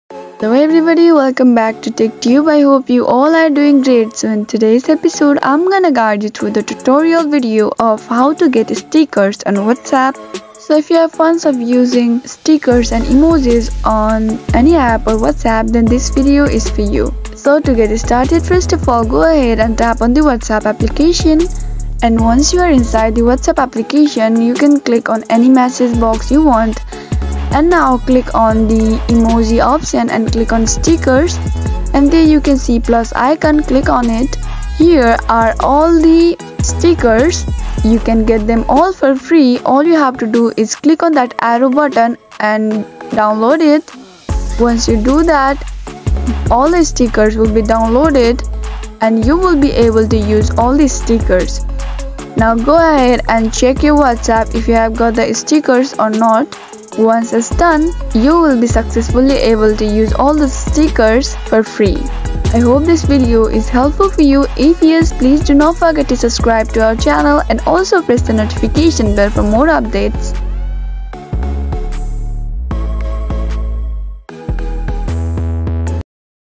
How to Get Stickers on WhatsApp (2023)
Learn How to Get Stickers on WhatsApp. Learn the easy steps to spice up your WhatsApp conversations with vibrant stickers! In this tutorial, we guide you through the process of adding and using stickers on WhatsApp, making your chats more expressive and fun.